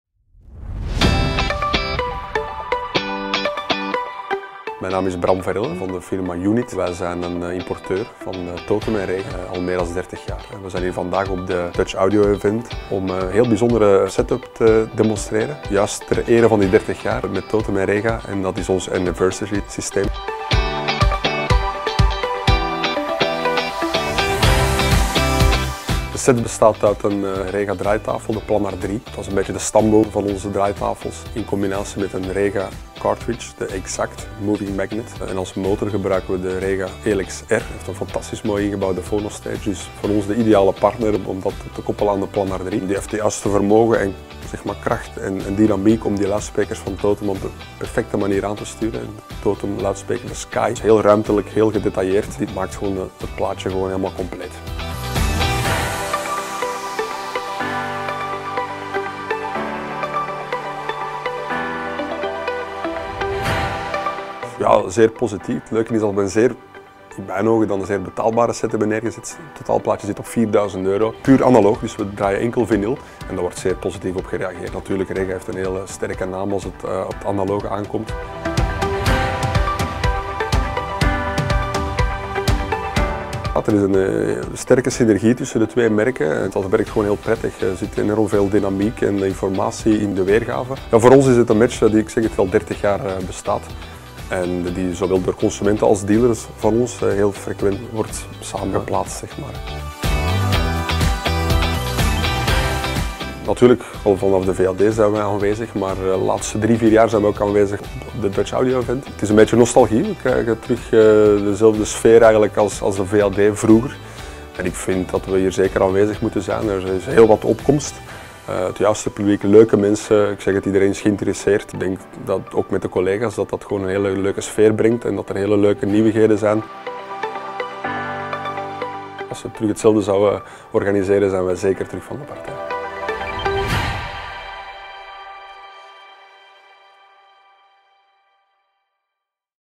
Sfeerverslag: Joenit op het Dutch Audio Event 2022
Je kan er niet omheen: Rega Research en Totem Acoustic maken al 30 jaar deel uit van Joenit’s merkenpakket. Om dat te vieren werd een jubileum set in de schijnwerpers gezet bestaande uit de Rega Elex-r geïntegreerde versterker met standaard ingebouwde phono amp, de Rega Planar 3 platendraaier met Rega Exact MM element en de Totem Acoustic Sky speakers. Deze opstelling werd eerder beschreven als volgt: een ‘verrukkelijke set’ zonder overbodige franjes voor verstokte liefhebbers van vinyl. Dat klonk als muziek in de oren. Benieuwd naar ons sfeerverslag?